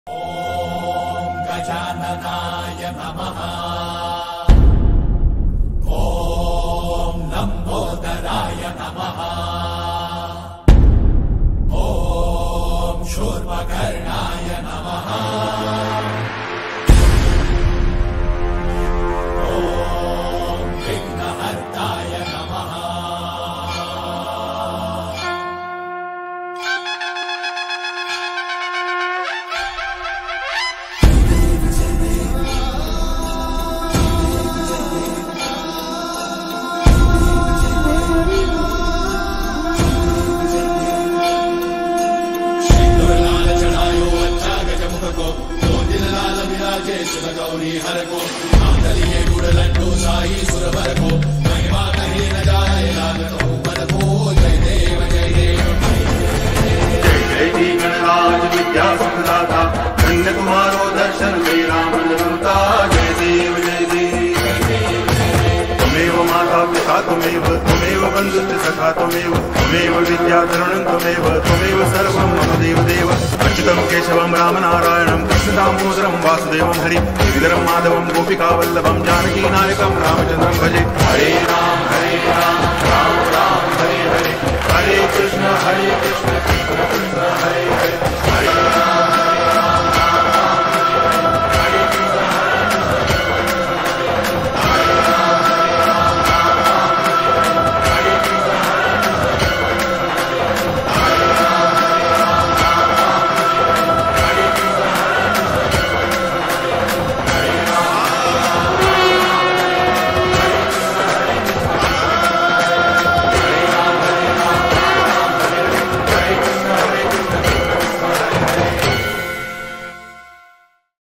Pran Pratishtha of Shree Ganesha, Shree Dhanvantri and Ma Saraswati
Some of the wonderful moments of Pran Pratishtha of Shree Ganesha, Shree Dhanvantri and Ma Saraswati at central campus, SAM College of Engineering and Technology.